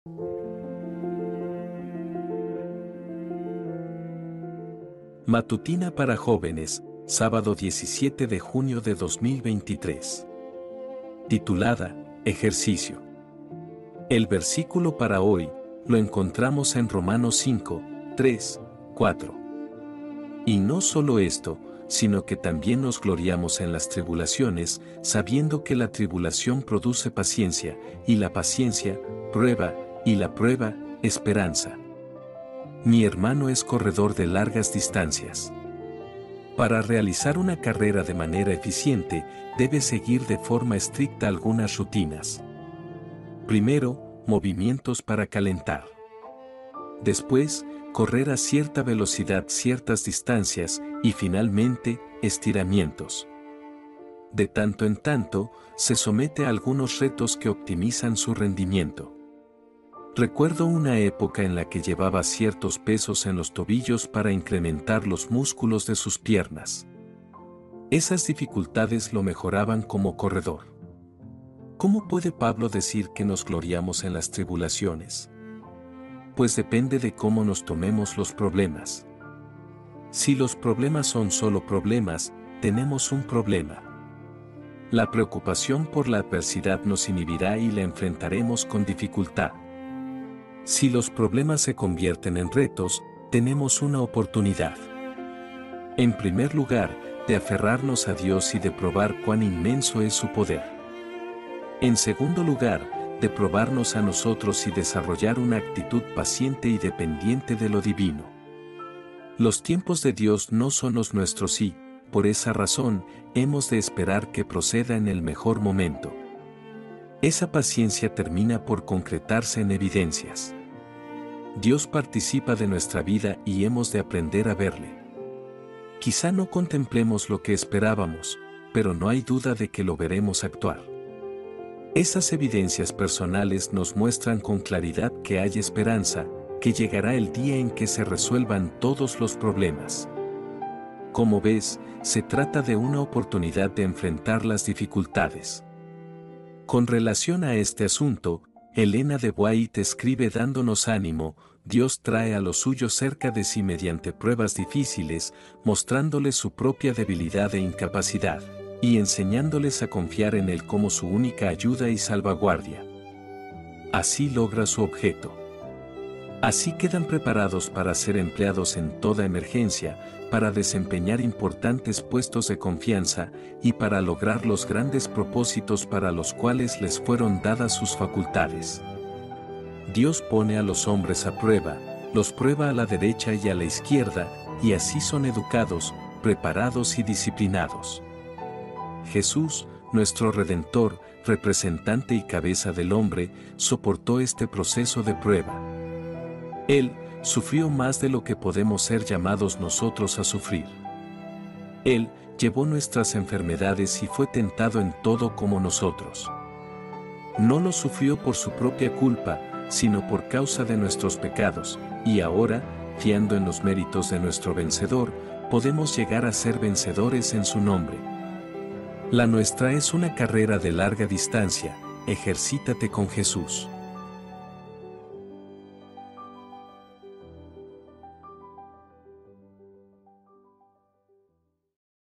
Ejercicio | Carácter - Devocional de Jóvenes | 17 de junio 2023
Ejercicio | Carácter - Devocional de Jovenes | 17 de junio 2023 | AD7Devocional |
Y no solo esto, sino que también nos gloriamos en las tribulaciones, sabiendo que la tribulación produce paciencia; y la paciencia, prueba; y la prueba, esperanza. Romanos 5:3, 4.
AUDIO usado con permiso de nuestros amigos:
SIGUE en Instagram el post de la matutina y el versículo diario:
Autor: Victor M Armenteros
Titulo: Carácter - Ser Como Jesus y Disfrutar de la Eternidad
Titulo: Palpitando la eternidad